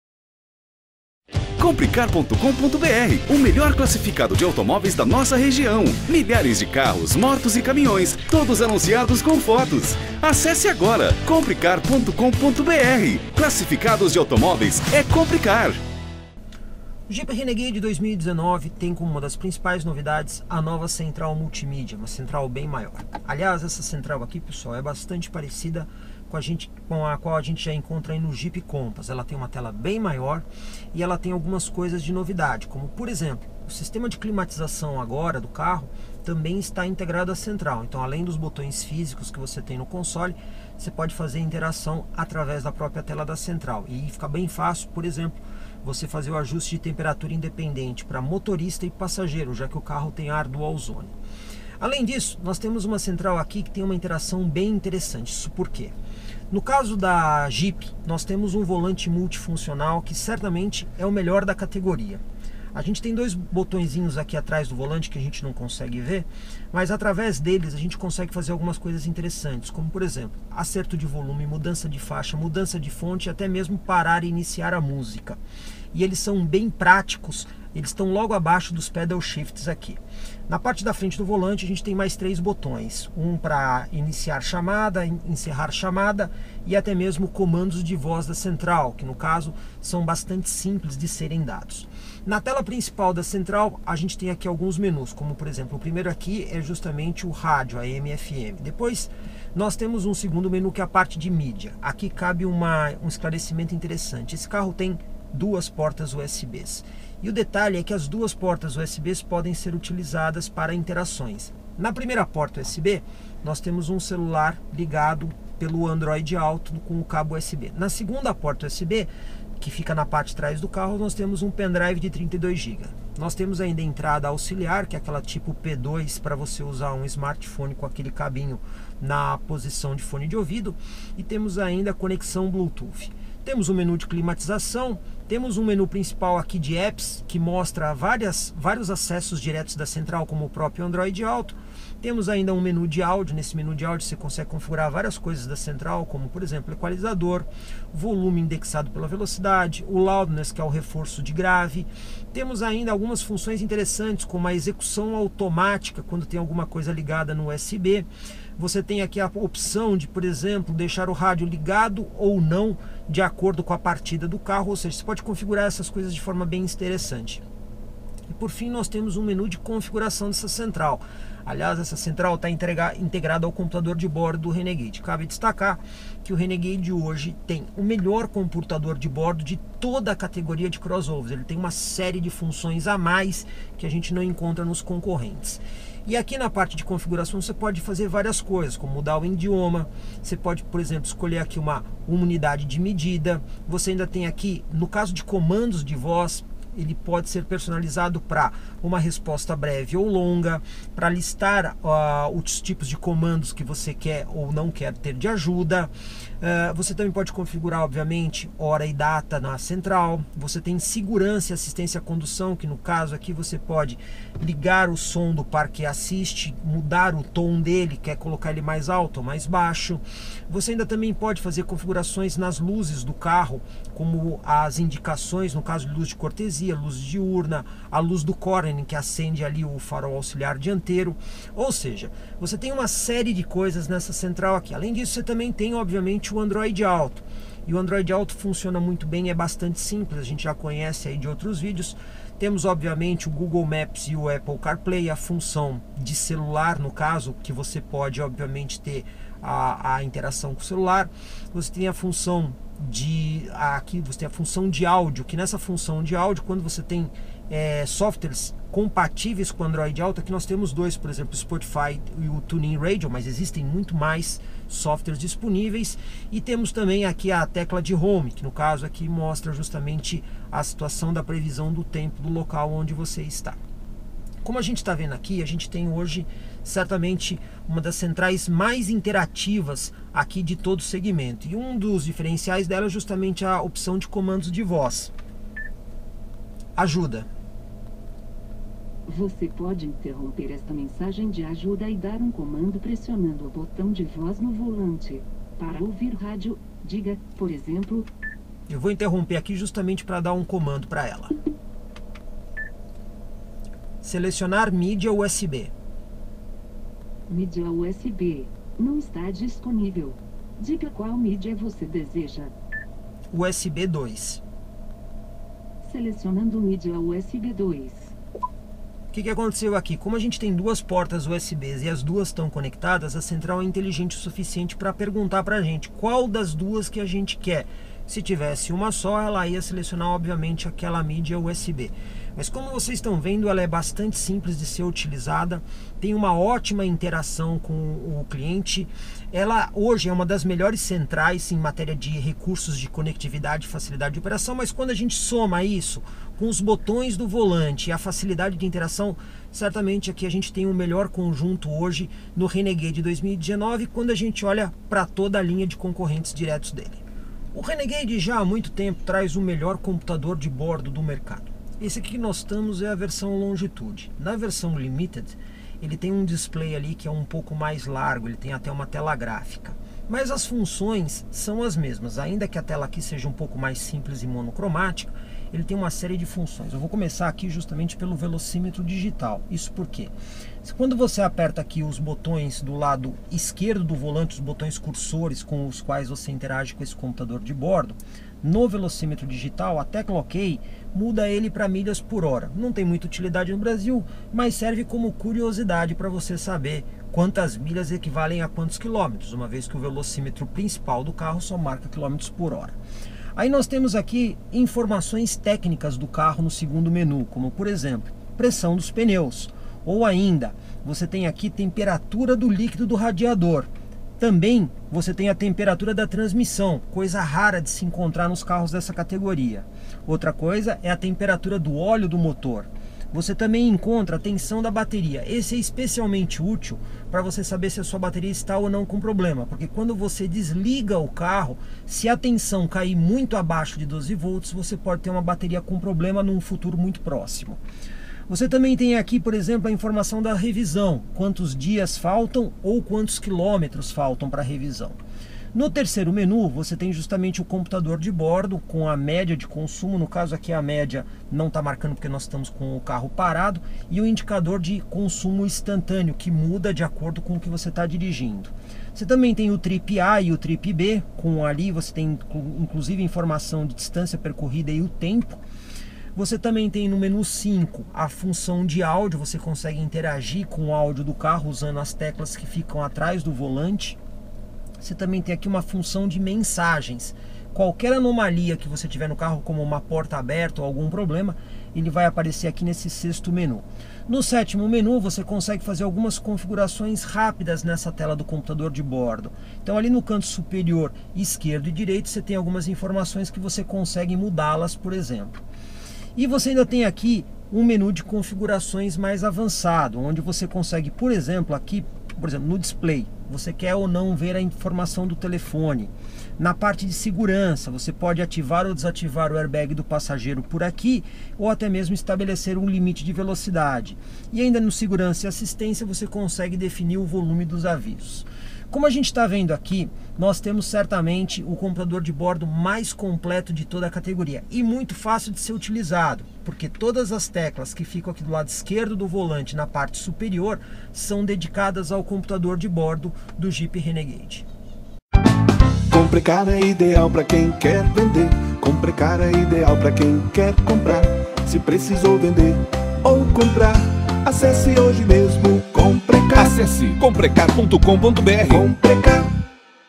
Jeep Renegade 2019 - Nova central multimídia
Na linha 2019 o Jeep Renegade ganhou uma nova central multimídia, com tela bem maior de 8.4'',  e novos recursos de conectividade, como Android Auto e Apple Carplay